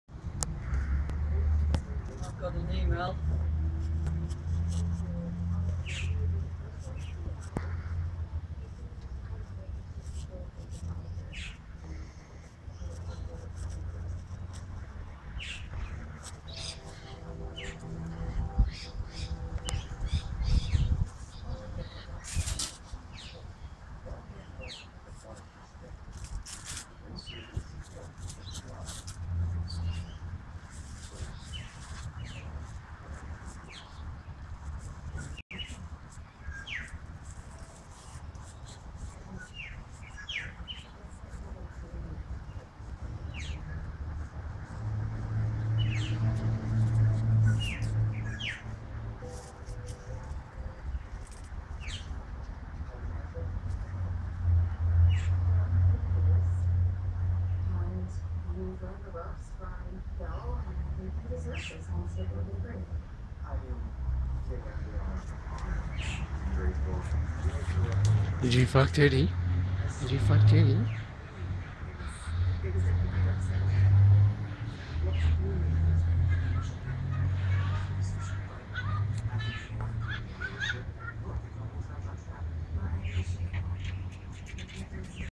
rainbow lorikeet, horny, sex, with his teddy all the way
Pet rainbow lorikeet horny parrot.